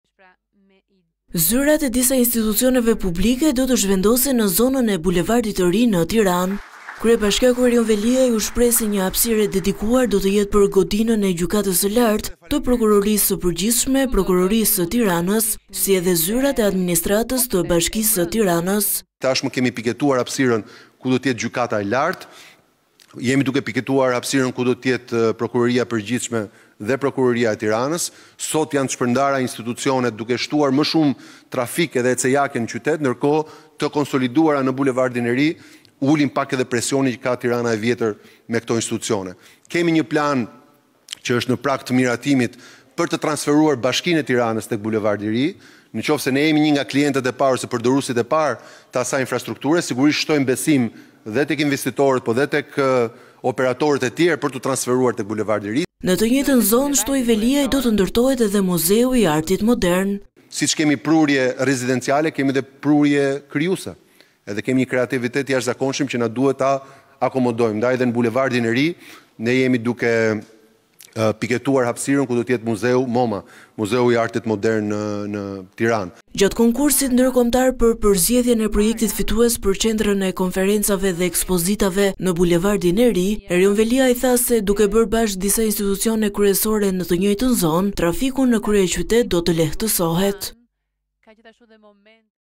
Veliaj tregon projektet e reja që do të ndërtohen te Bulevardi i Ri - Vizion Plus
Gjatë fjalës së tij, në konkursin ndërkombëtar për përzgjedhjen e projektit fitues për Qendrën e Konferencave dhe Ekspozitave në Bulevardin e Ri, kryetari i Bashkisë së Tiranës, Erion Veliaj tregoi edhe disa institucione të tjera që do të vendosen në atë zonë. Ato do të jenë godinat e Gjykatës së Lartë dhe Muzeut i Artit Modern, përfshirë këtu edhe një ndërtesë të re administrative e Bashkisë së Tiranës. “Disa prej institucioneve të drejtësisë do të kalojnë te Bulevardi i Ri. Kemi piketuar hapësirën ku do të jetë godina e re e Gjykatës së Lartë dhe jemi duke piketuar hapësirën për Prokurorinë e Përgjithshme dhe Prokurorinë e Tiranës. Sot institucionet janë të shpërndara, çka shton trafikun dhe ecejaket në qytet, ndërkohë duke i sjellë në Bulevardin e Ri ulin pak presionin që ka Tirana e vjetër. Kemi një plan në prag të miratimit për të zhvendosur zyrat administrative të Bashkisë së Tiranës te Bulevardi i Ri. Nëse jemi ne të parët që zhvendosemi aty, shtojmë besimin te investitorët dhe operatorët e tjerë për t’u transferuar edhe ata te Bulevardi i Ri. Godina aktuale e Bashkisë së Tiranës do të ruajë selinë tradicionale, ku do të bëjmë pritjet dhe përcjelljet protokollare. Ndërkohë, administrata do të transferohet në një ndërtesë ultra moderne te Bulevardi i Ri”, tha Veliaj. Ai u shpreh se në Bulevardin e Ri do të ndërtohet edhe Muzeu i Artit Modern. “Tirana, si Kryeqyteti Europian i Rinisë vitin e kaluar, dhe sot i Qyteti Europian i Sportit, e nesër potencialisht i Kulturës, ka nevojë të shtojë hapësirat kulturore. Sado që Galeria e Arteve po rikonstruktohet dhe do të shtohet në volum, sado që Tirana ka hapur një Galeri të qytetit, për të promovuar talentet e reja, nuk mjafton. Siç kemi prurje rezidenciale, kemi edhe prurje krijuese, kemi një kreativitet të jashtëzakonshëm që na duhet ta akomodojmë në hapësira të reja. Në Bulevardin e Ri jemi duke piketuar hapësirën që do jetë muzeu ‘Moma’, Muzeu i Artit Modern në Tiranë. Ndërkohë, jemi duke bërë një plan për një shesh ceremonial, në fund të Bulevardit të Ri, siç është sheshi Nënë Tereza në krahun tjetër”, bëri me dije Veliaj.